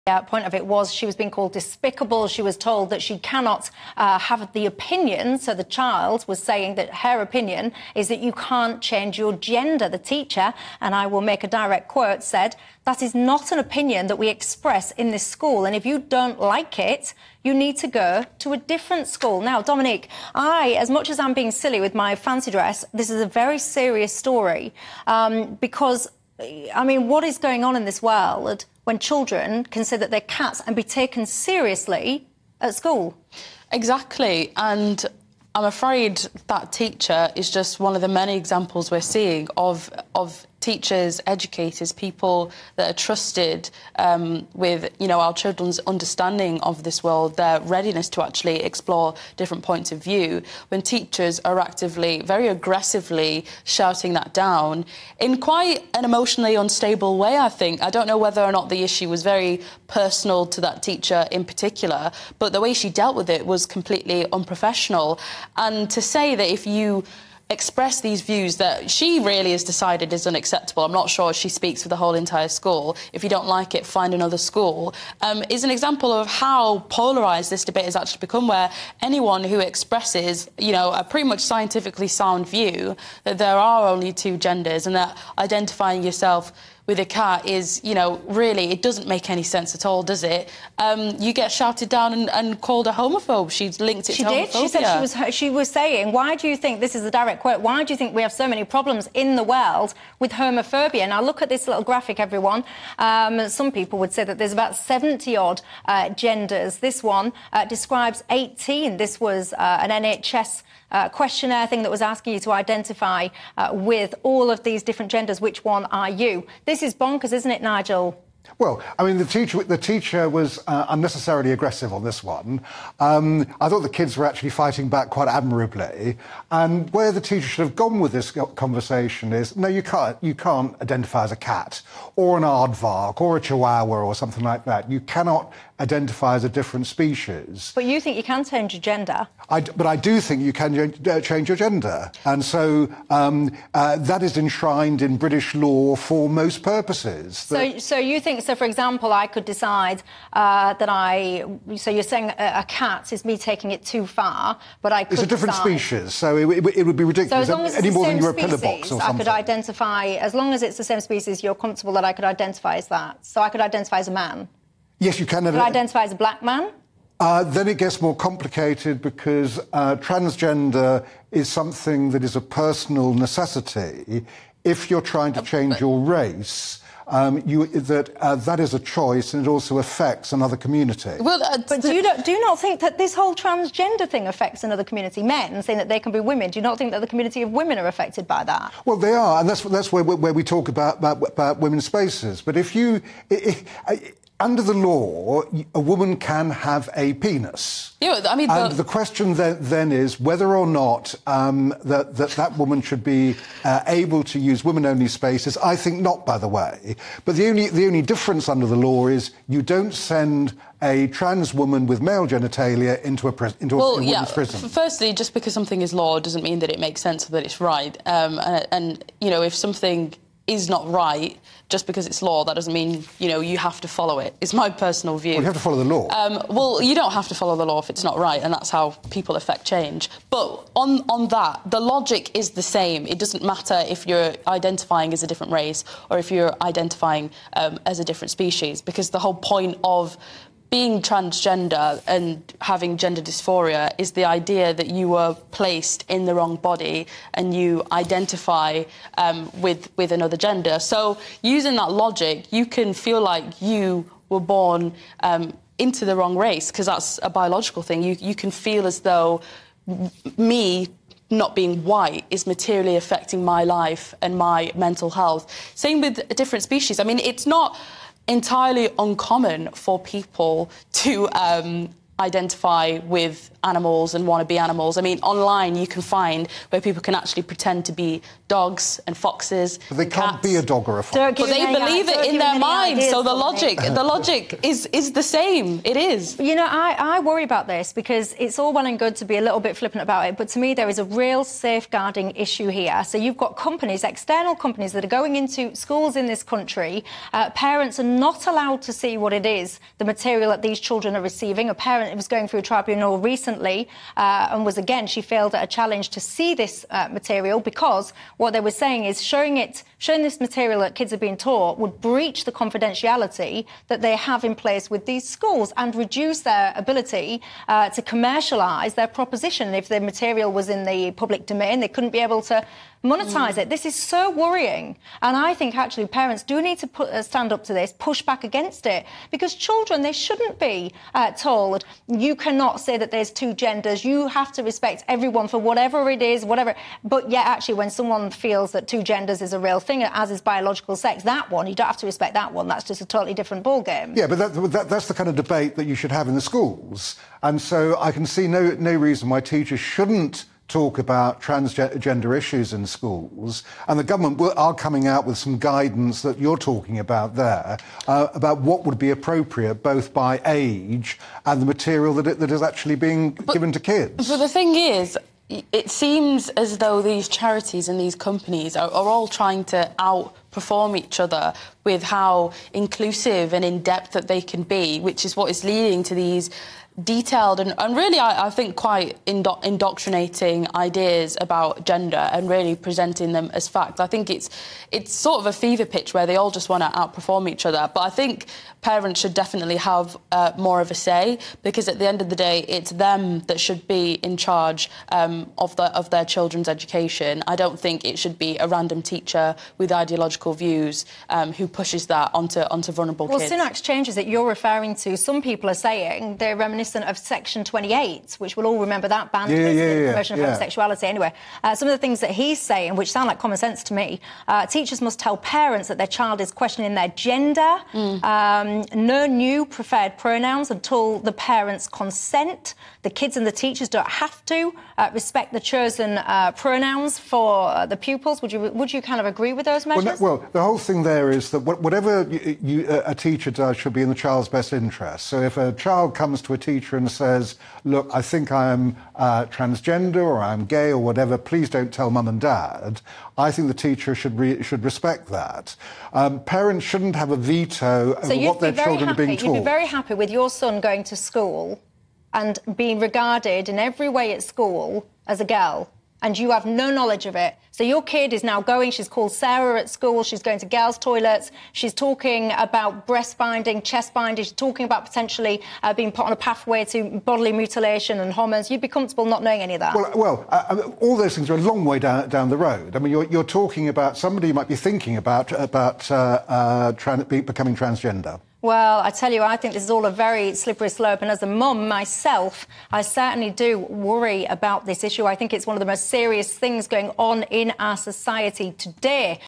Girl branded ‘despicable’ for questioning cat-identity of other pupil | ‘What is going on?!’
‘What is going on in this world when children can say that they are cats and be taken seriously?’

Michelle Dewberry reacts to a schoolgirl being branded ‘despicable’ for questioning the cat-identity of another pupil.